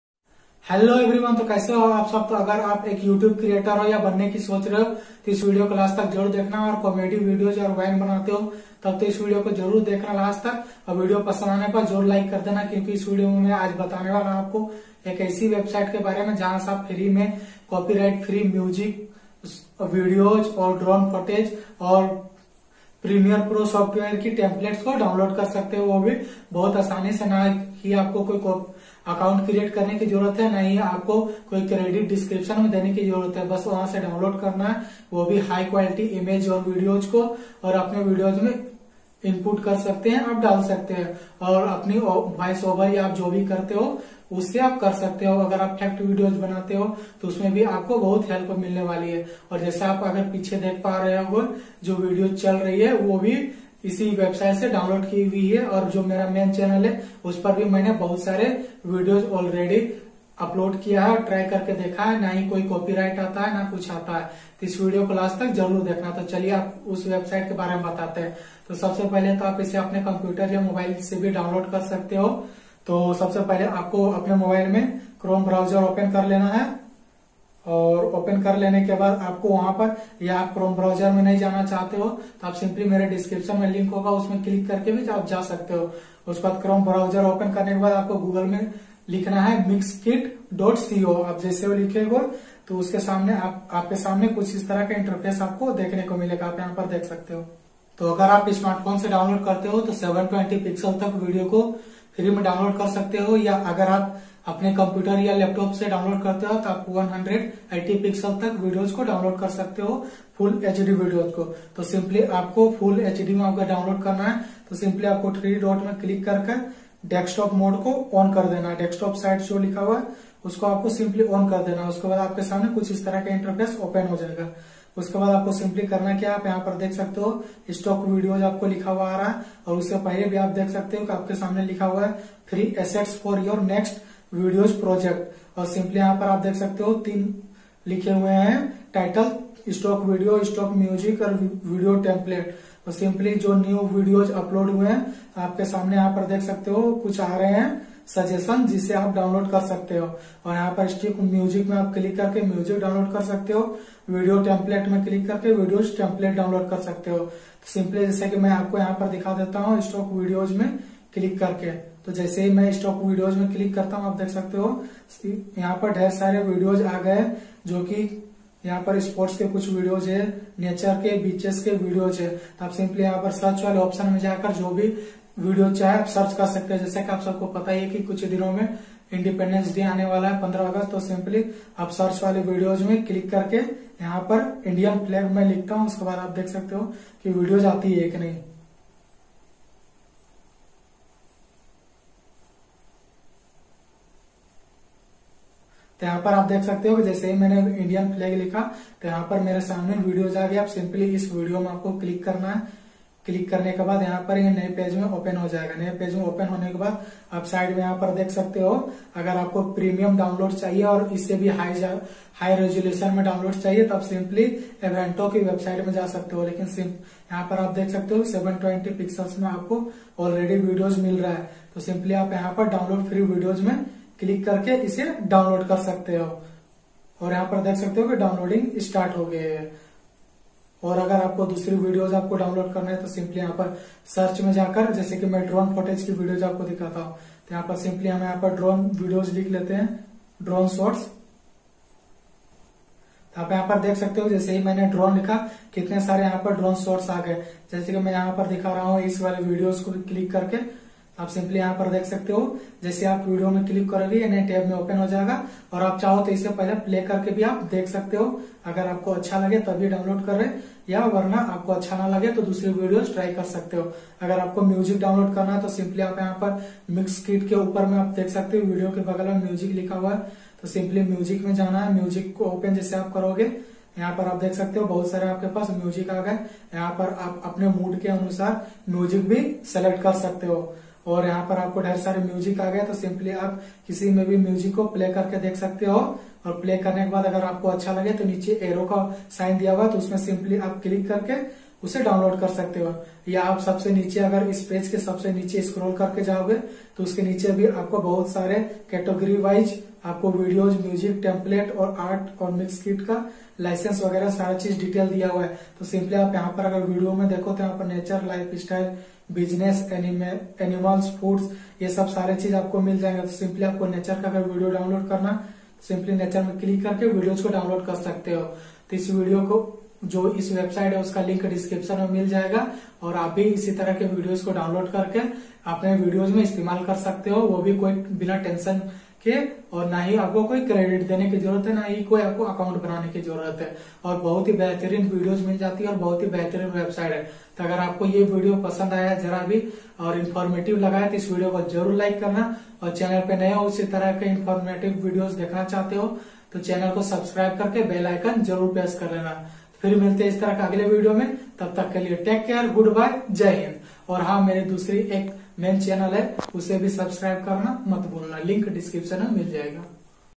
How to Download Copyright Free Music, Videos & Image for YouTube | Copyright Free Video Clips
Dosto Aaj Ki Is Video Me Maine Aapko Ye Bataya Hai Ki Apni YouTube Videos Ke Liye Copyright Free Music, Video Clips And Image Kaise Download Kar Sakte Hai Jis Se Aapko Koi BHi Copyright Strike Nahi Milega Thank you so much...

LINKS

*PRODUCT
  PURCHASE